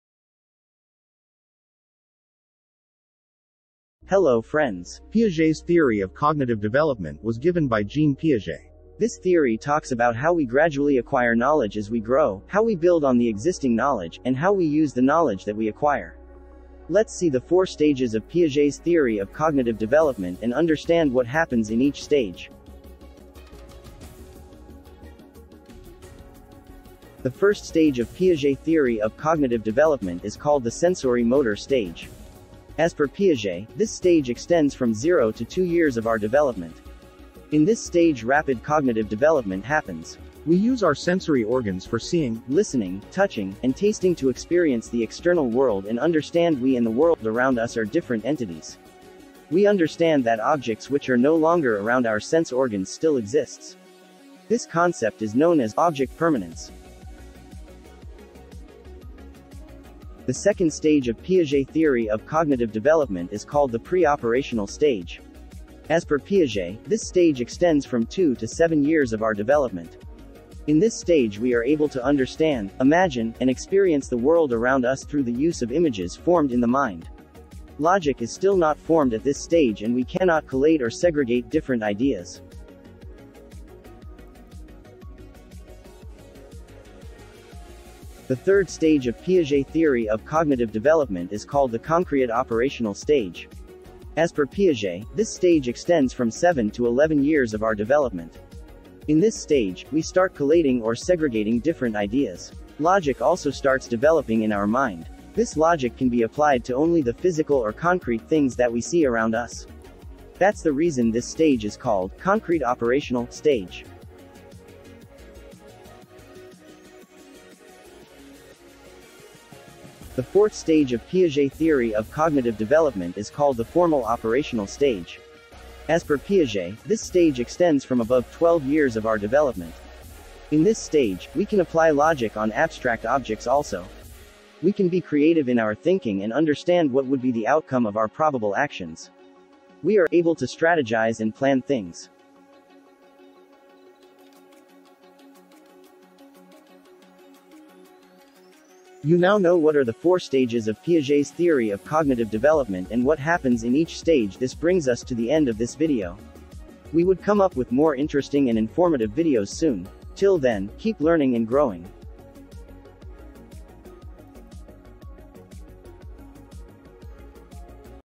Piaget Theory of Cognitive Development | Cognitive Development Theory by Jean Piaget
Piaget's theory of cognitive development was given by Jean Piaget. This theory talks about how we gradually acquire knowledge as we grow, how we build on the existing knowledge, and how we use the knowledge that we acquire.

00:00:05 - Who gave Piaget's theory of cognitive development?
00:00:09 - What is Piaget's theory of cognitive development?
00:00:27 - Stage 1: Sensorimotor stage
00:01:00 - Stage 2: Pre-operational stage
00:01:27 - Stage 3: Concrete operational stage
00:02:00 - Stage 4: Formal operational stage